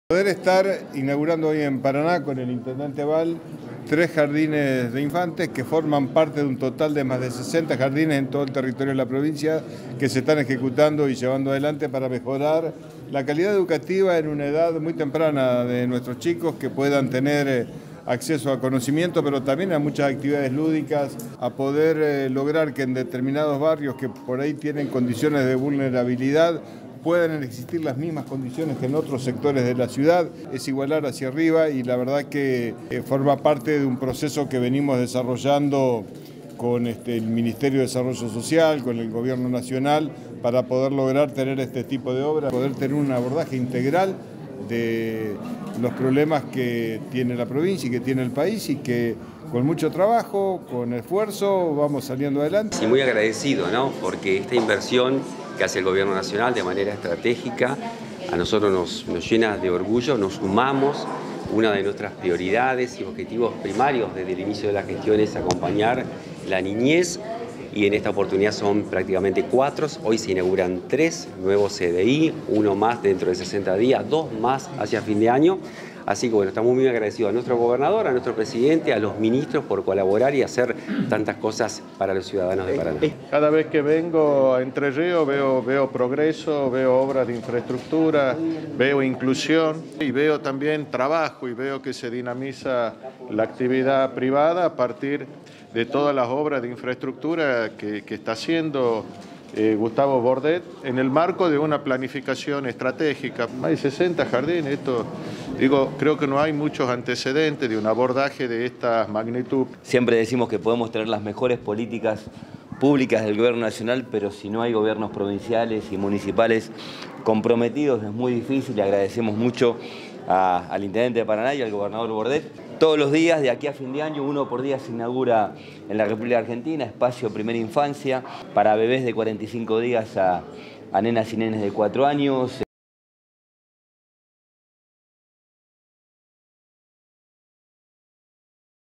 Bahl acompañó a Bordet y a funcionarios nacionales en su recorrida por Paraná
Entre otras actividades, se inauguró en Paraná la puesta en valor de tres espacios de primera infancia. Estuvieron presentes el gobernador Gustavo Bordet junto al jefe de Gabinete, Juan Manzur, el ministro de Desarrollo Social de Nación, Juan Zabaleta, la vicegobernadora Laura Stratta y el intendente Adán Bahl.